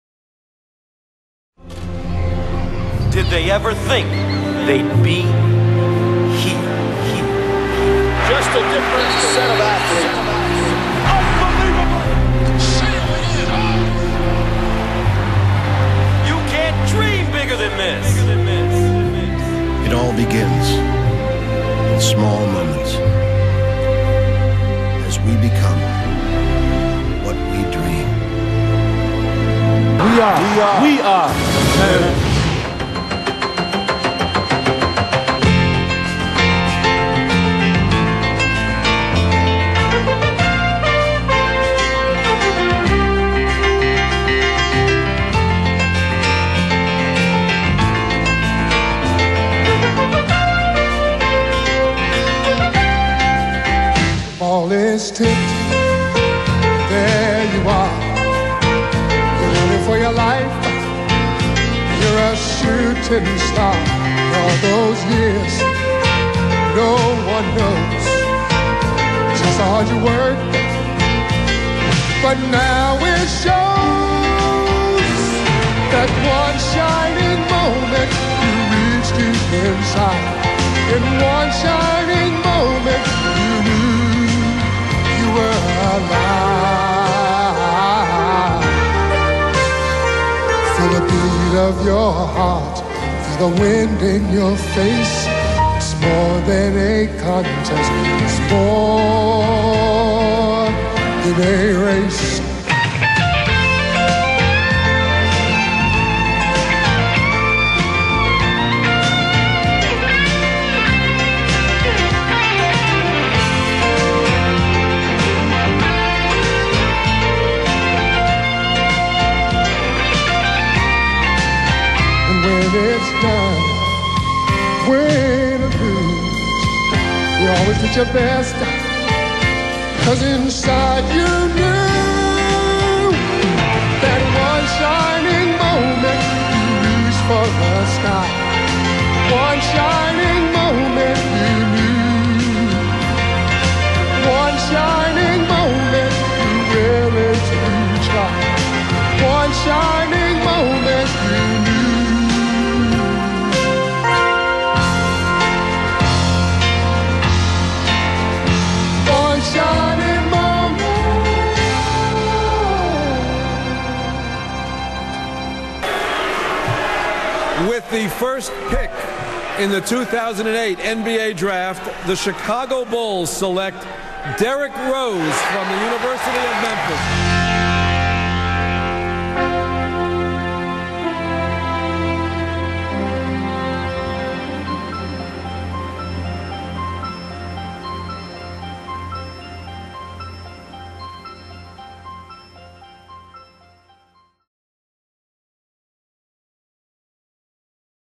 Memphis Madness Highlight Video 2008
Memphis Tiger Basketball Highlight video that played during 2008 Memphis Madness.  Video work courtesy of Hugh Greer.